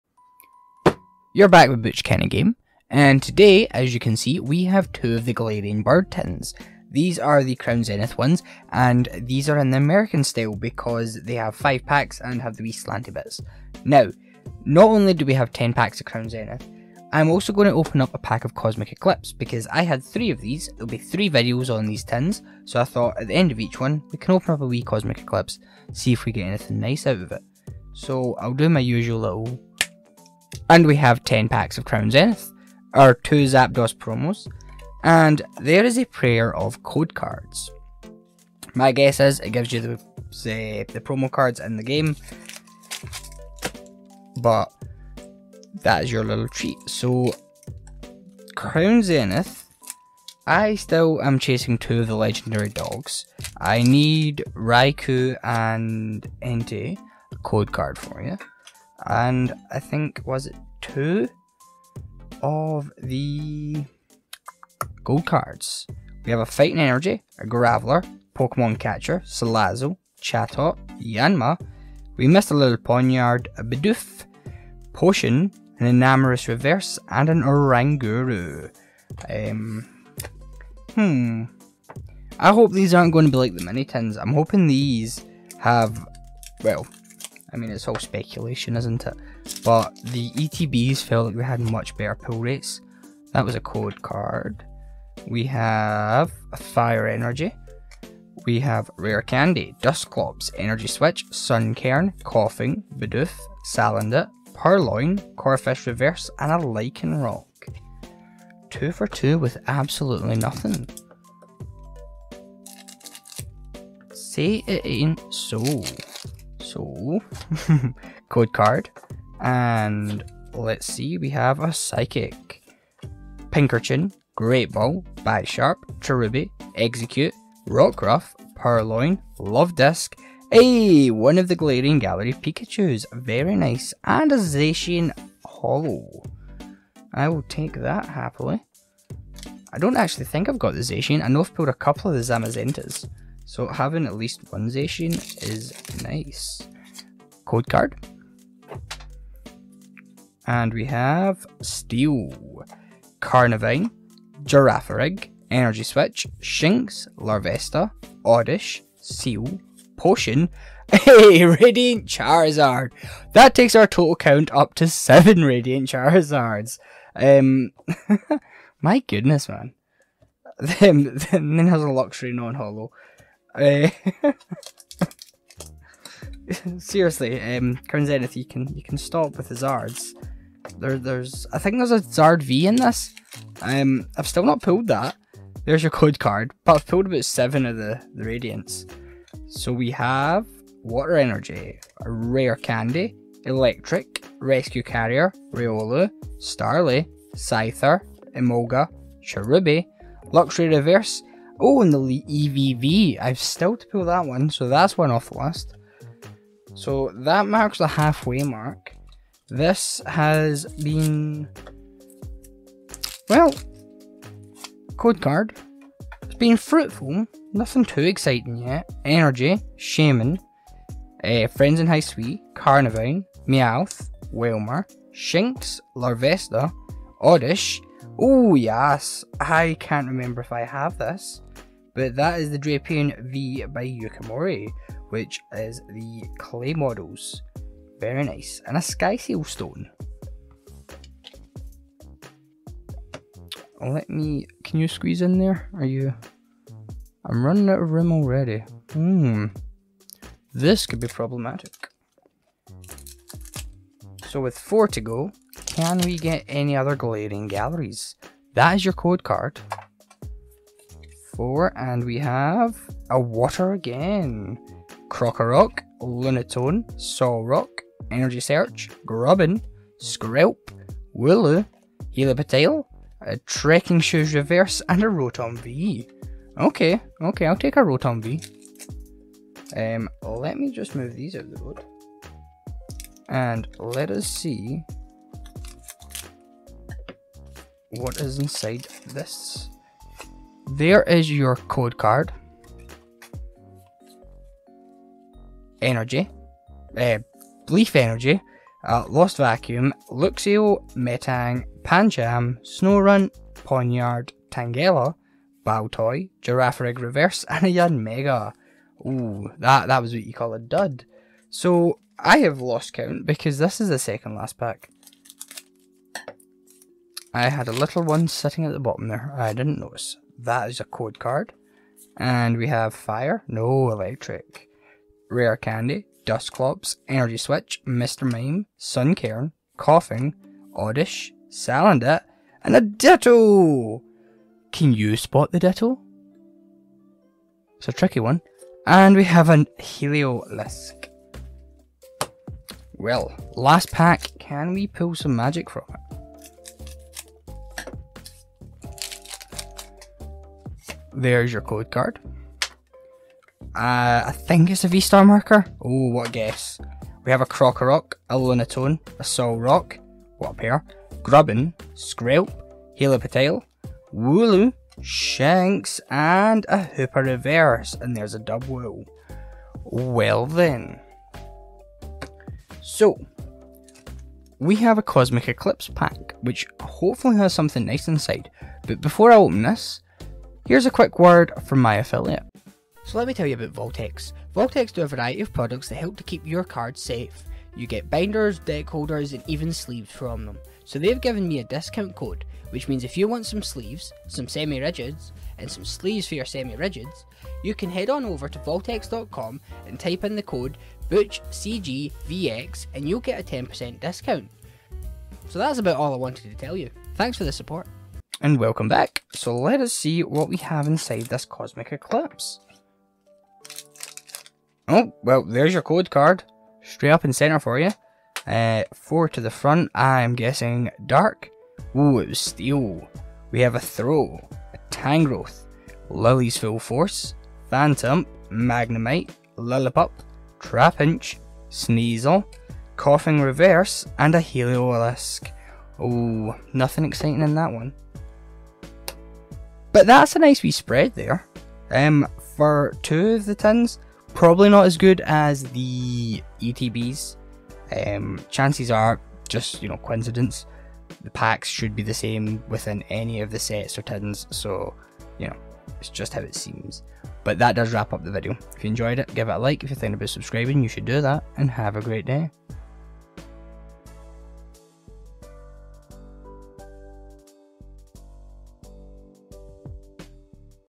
Will These Galarian Zapdos Tins Be Shockingly Good?
In this video, I open two Pokemon Crown Zenith - Galarian Zapdos Tins! These tins come with an awesome promo card and 5 packs of Crown Zenith.

Join me as I crack open two of these tins. Will I pull any rare cards? You'll have to watch to find out!

VAULTX DISCOUNT
At checkout use the Code: BUTCHCGVX

Why not check out some of my other openings?

I Opened A Shining Fates Elite Trainer Box And Got How Many Shiny Vault Pokémon Cards? -

CREDITS